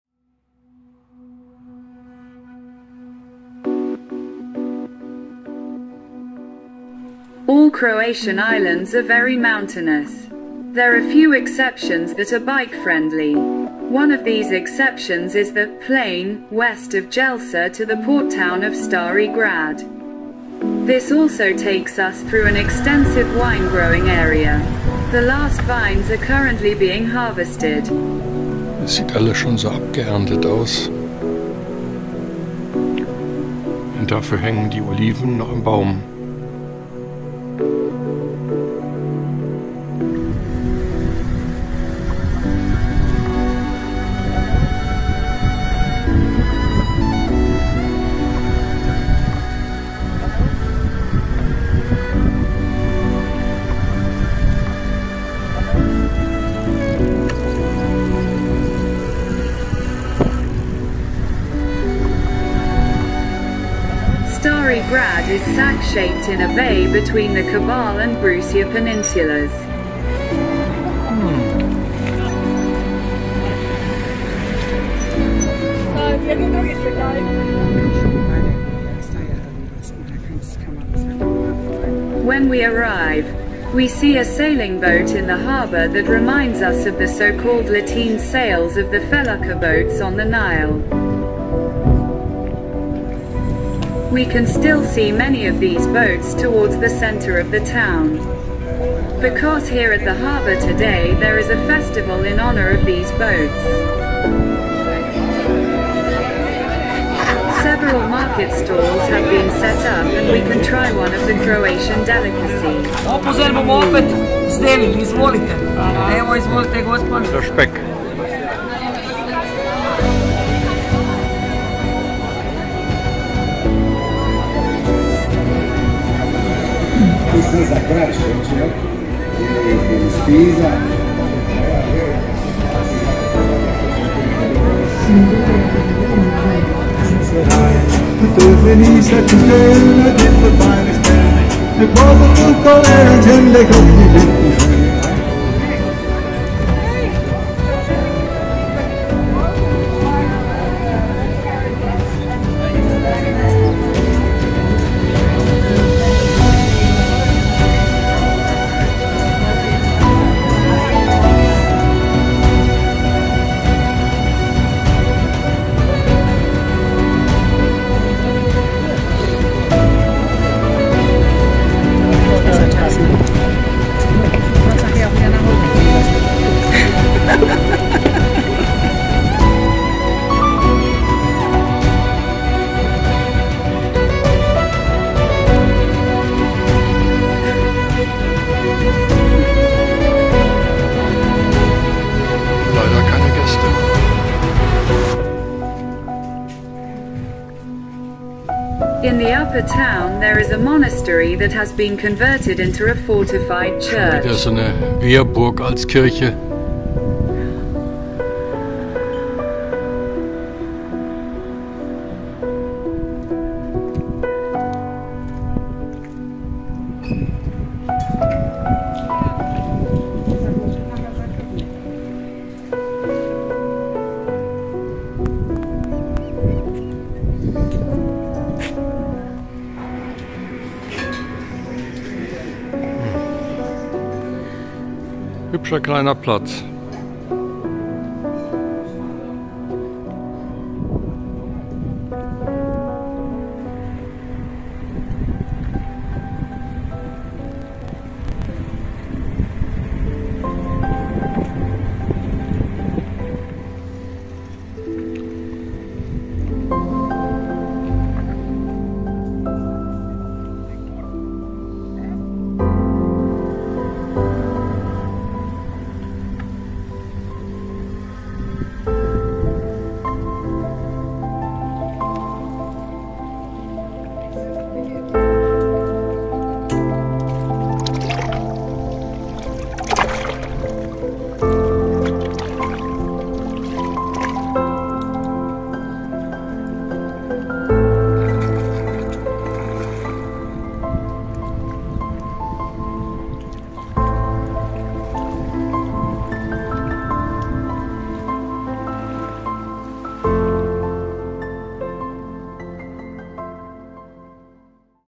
202409_Croatia in Autumn 2024 - Part-8: Stari Grad on the island of Hvar
All Croatian islands are very mountainous. There are few exceptions that are bike-friendly. One of these exceptions is the "plain" west of Jelsa to the port town of Stari Grad. This also takes us through an extensive wine-growing area. The last vines are currently being harvested.

Stari Grad is sack-shaped in a bay between the Kabal and Brusje peninsulas. When we arrive, we see a sailing boat in the harbor that reminds us of the so-called lateen sails of the felucca boats on the Nile. We can still see many of these boats towards the center of the town. Because here at the harbor today there is a festival in honor of these boats. Several market stalls have been set up and we can try one of the Croatian delicacies. In the upper town there is a monastery that has been converted into a fortified church. Sept 2024